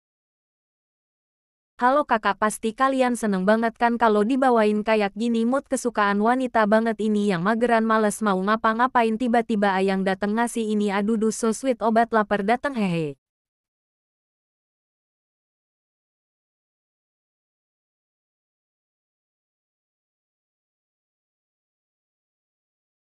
Tiba tiba ayang dateng ngasih makanan minuman gini aduduh sweet banget sih.
Hallo kakak pasti kalian seneng banget kan kalo dibawain kayak gini mood kesukaan wanita banget ini yang mageran males mau ngapa ngapain tiba tiba ayang dateng ngasih ini aduduh so sweet obat laper dateng hehe